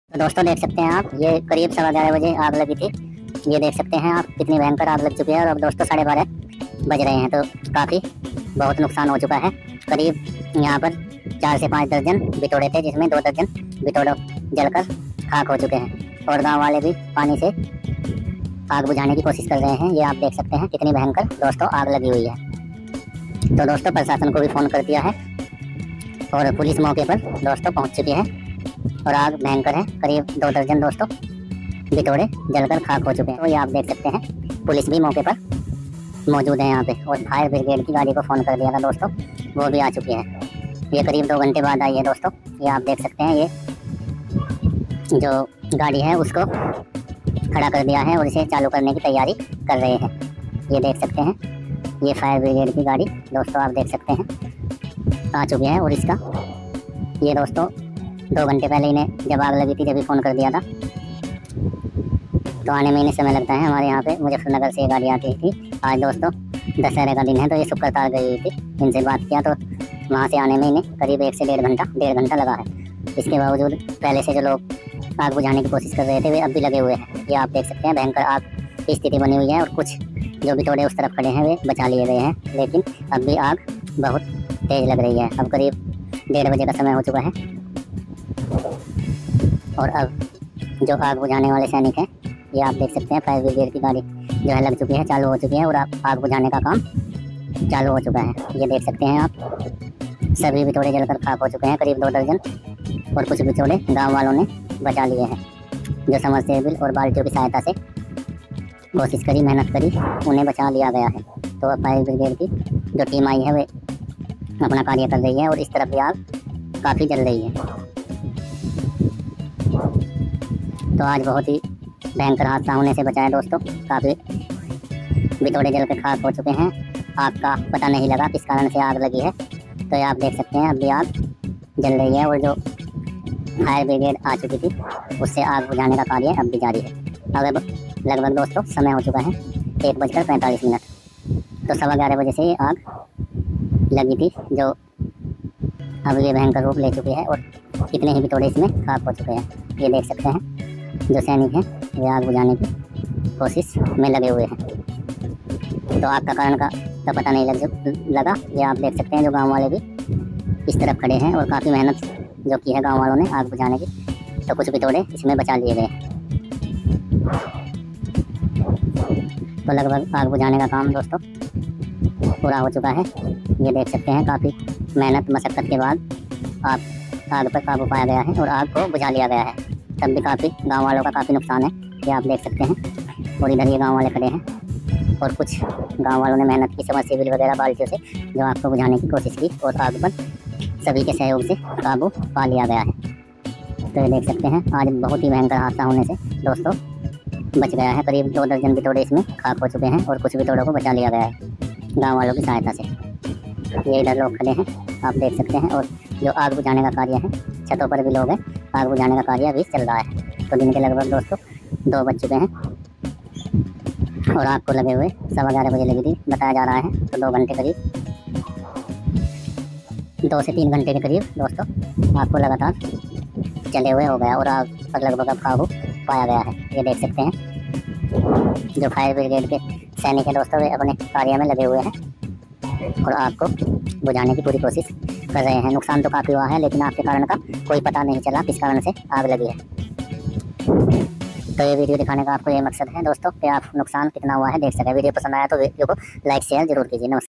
मेरे गांव मैं बीटोड़ो में लगी आग काफी नुकसान फायर ब्रिगेड बुलाना पड़ा
gaon mein lagi Aage kafi nuksan

gaon ka vlog
Delhi vlog
video
veer vlogs one